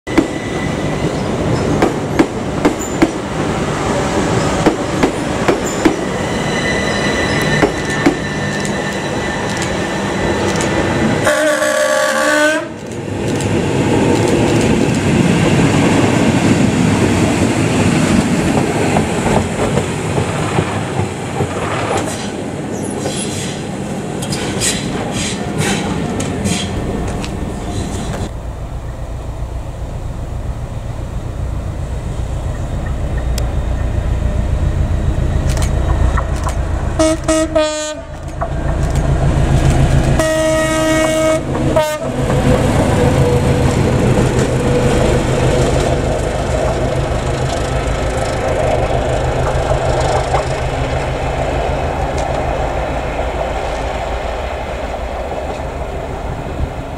Southern 73202 Working Driver Training Runs @ Barnham - 2/8/19
This Video: Southern 73202 Working Driver Training Runs @ Barnham. On 2nd August 2019 Southern Ran Their Class 73 To Bognor Regis To Train Drivers For Future Thameslink Services Along The Route. The Class 73 Is Seen Twice, Firstly Working 0Y41 Stewarts Lane-Bognor Regis Then Returning With 0Y30 Bognor Regis-Three Bridges Tilegate Sidings. Thanks To The Driver For The Tones Both Times.